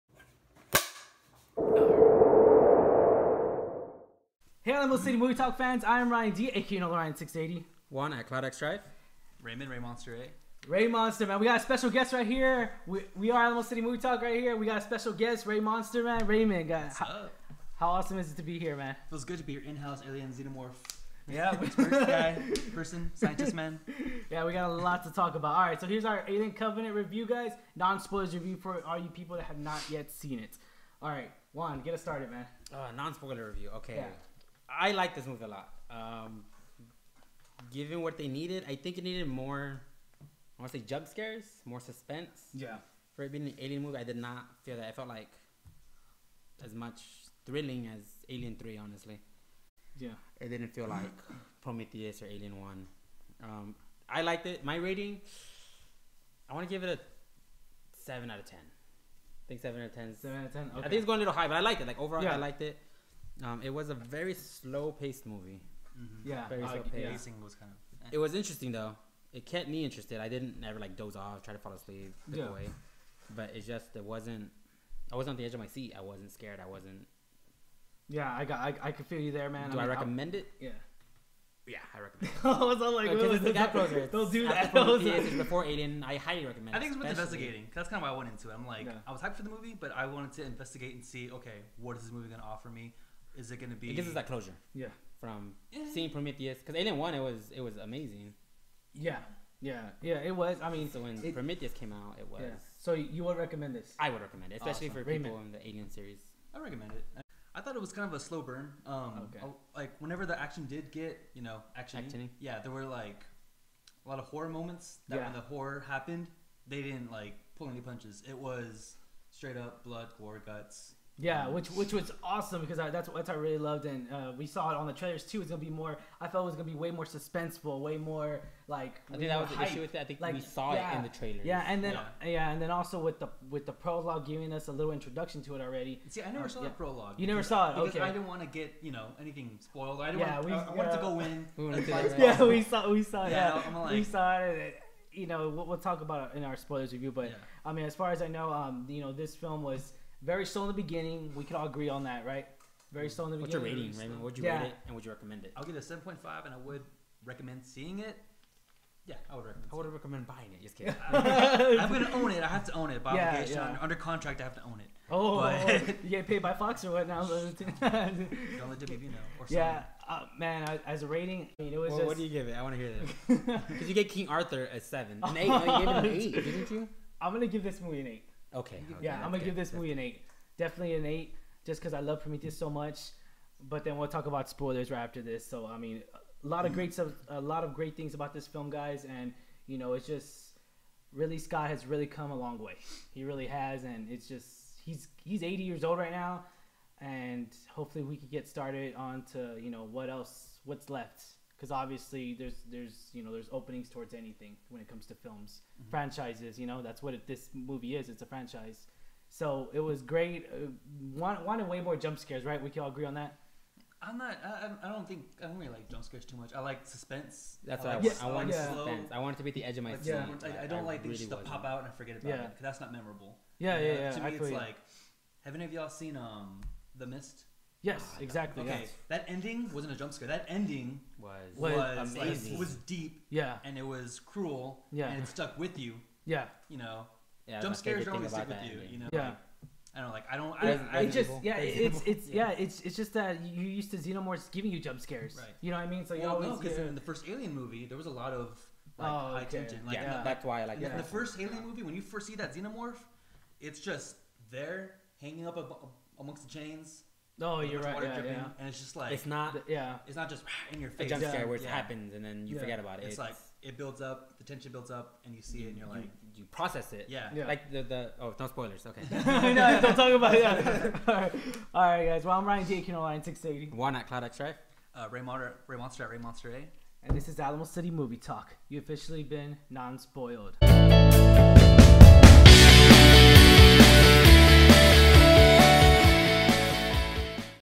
Alien: Covenant Non-Spoilers Review
The Alamo City crew gets right into detail on the NEW Alien: Covenant Non-Spoilers Review. Also Special Guest Raymond  aka Raymonster_ay.
As Always comment on below if you disagree or agree or if you want to show some love!